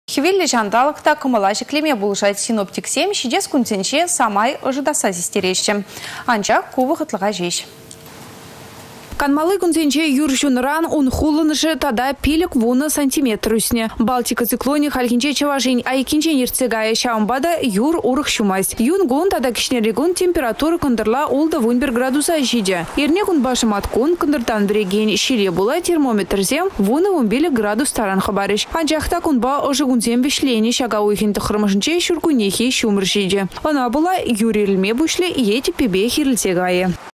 Çанталăк вăхăтлăха ăшăтать
Хĕвеллĕ çанталăк та кăмăла çĕклеме пулăшать, синоптиксем çитес кунсенче самай ăшăтасса систереççĕ, анчах ку вăхăтлăха çеç.